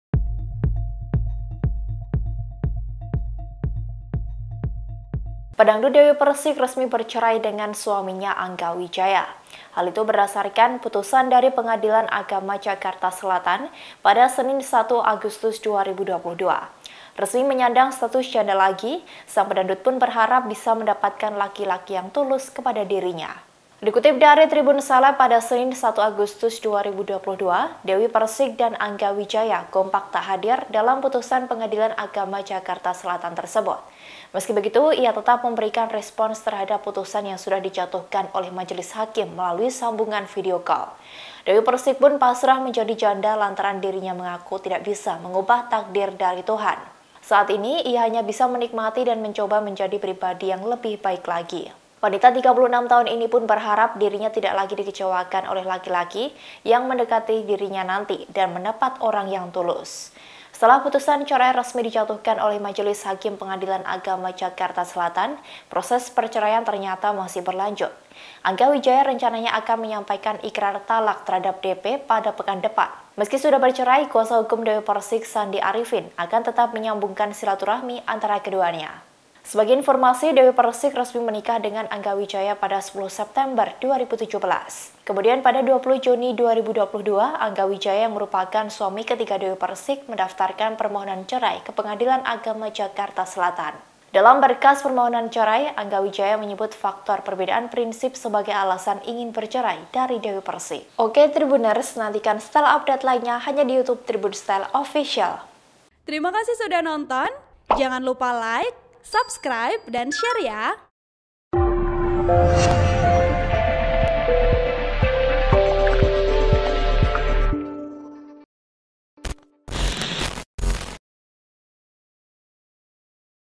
Tok! Dewi Perssik & Angga Wijaya Resmi Bercerai, sang Pedangdut Berharap Dapat Laki-laki yang Tulus
TRIBUN-VIDEO.COM - Pedangdut Dewi Perssik resmi bercerai dengan suaminya, Angga Wijaya. Hal itu berdasarkan putusan dari Pengadilan Agama (PA) Jakarta Selatan pada Senin (1/8/2022).

Resmi menyandang status janda lagi, sang pedangdut pun berharap bisa mendapatkan laki-laki yang tulus kepada dirinya.